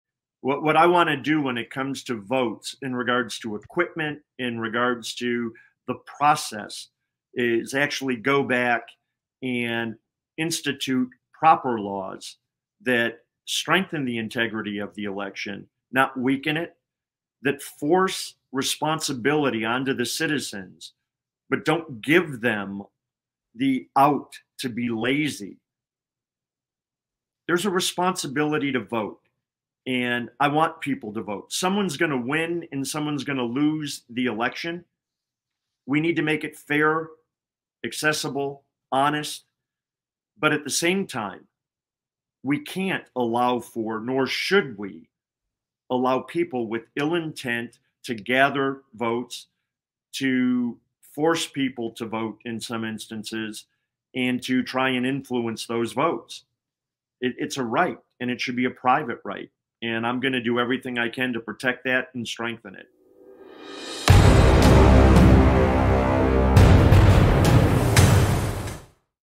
How do we keep ill intent out of our elections?
Hear Kevin Rinke's answer to this question and more by watching his full interview

And be sure to listen to and SHARE ALL 5 candidate interviews before making your decision for Michigan's Republican gubernatorial candidate on August 2nd!